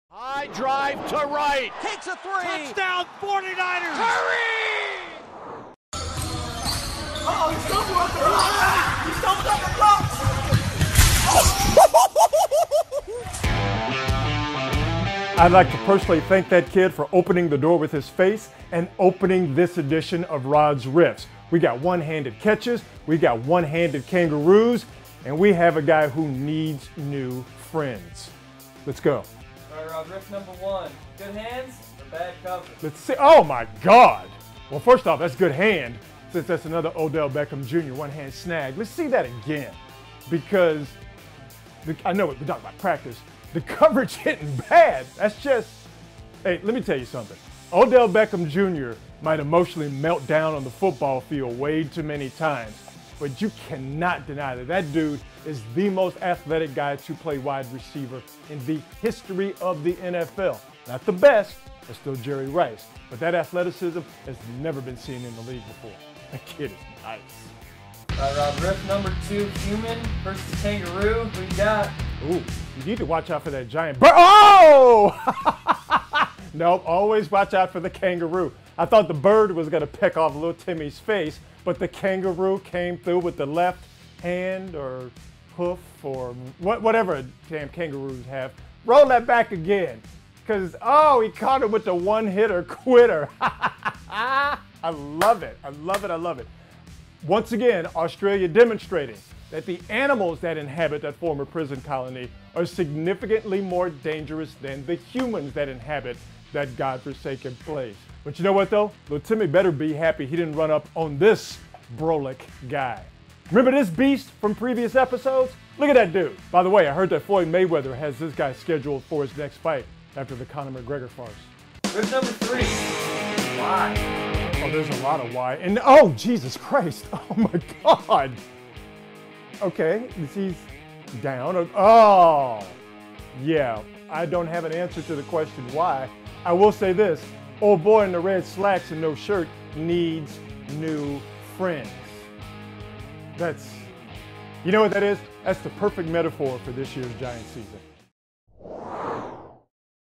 Rod's Riffs: A kid fights a Kangaroo and a guy needs some new friends
Rod is back with his weekly installment of Rod's Riffs. Today we take a look at Odell Beckham Jr.'s amazing catch, a kid eating a punch from a kangaroo and a guy who desperately needs some new friends.